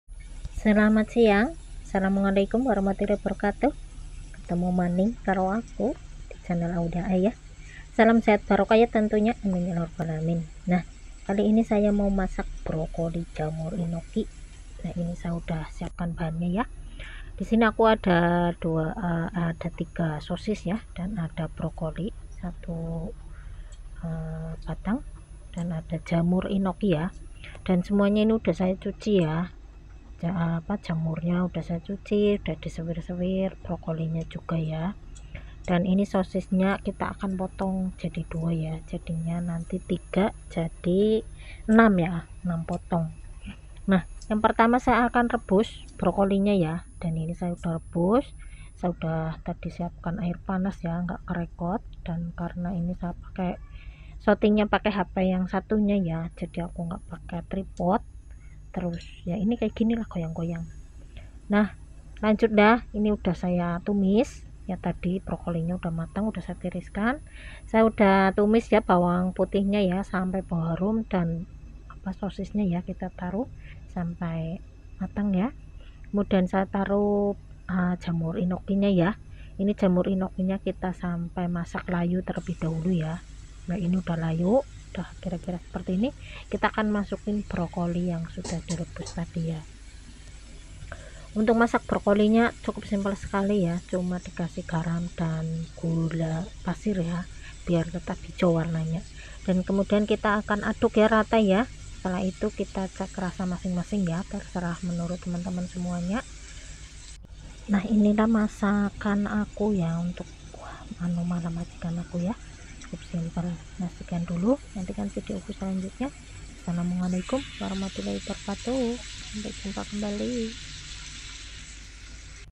Cuma 3 Bahan Jadi lauk yang enak sekali, kalian wajib coba
Assalamu'alaikum sahabat ku, jangan bingung dengan masaka  atau lauk apa buat makan. Nah disini ada 3 bahan bisa jadi lauk atau jadi menu yang enak. Teman teman boleh dengan bahan yang lain atau seadanya bahan yang ada dirumah /yang ada dikulkas.
Nah untuk selengkapnya , simak dan tonton cara aku masak 3 bahan ini , jangan diskip biar tidak gagal faham. Semoga bermanfaat
Selamat mencoba.